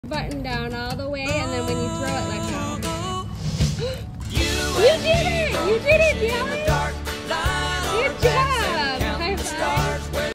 Life with cerebral palsy..First time casting!🎣
This is life with cerebral palsy first time casting ever! This was such a fun day! So proud

Wasco, IL 60183

Follow Discover with Dallas!

Founder of The Recess Project Inc. 🛝 which is non profit organization that helps build inclusive playgrounds for schools🏫